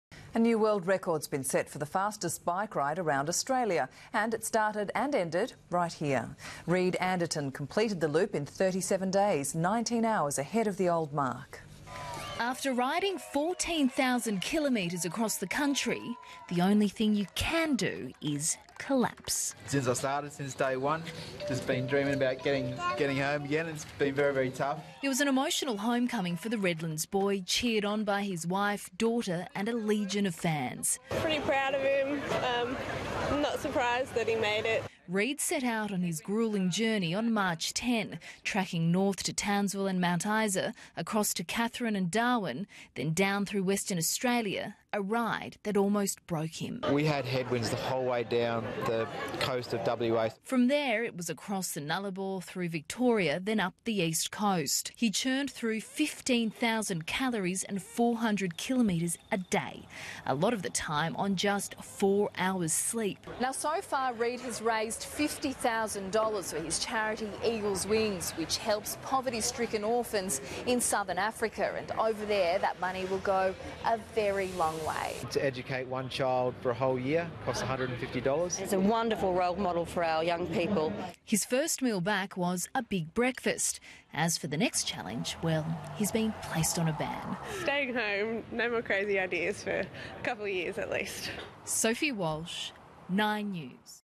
New world record set by biking around Australia
A new world record has been set for the fastest bike ride around Australia.

New world record set by biking around Australia watched by Todayvideos999.